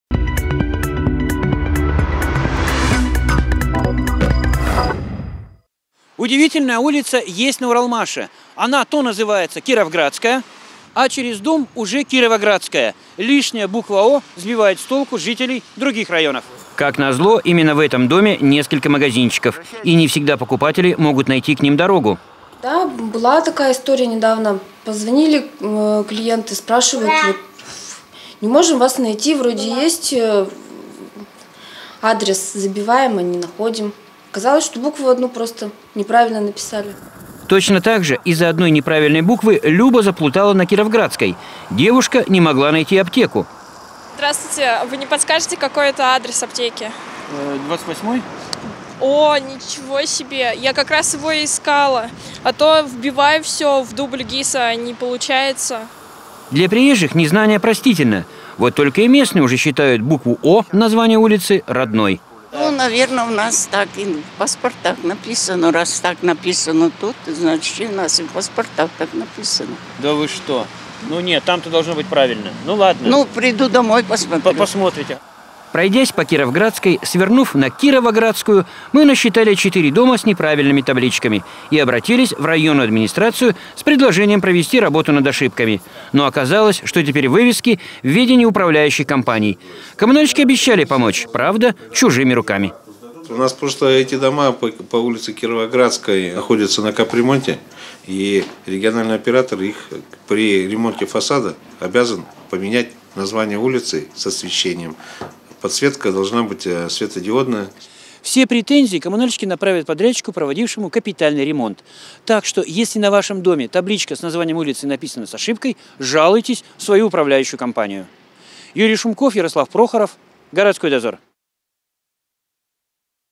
Две улицы в одной на Уралмаше / Новости
Две улицы в одной: ошибка на табличках путает жителей Уралмаша

Часть Кировградской стала Кировоградской

Лишняя буква "О" сбивает с толку местных жителей и приезжих из других районов. Пройдясь по Кировградской, свернув на Кировоградскую, мы насчитали четыре дома с неправильными табличками и обратились в районную администрацию с предложением провести работу над ошибками. Но, оказалось, что теперь вывески в ведении управляющих компаний. Коммунальщики обещали помочь, правда, чужими руками.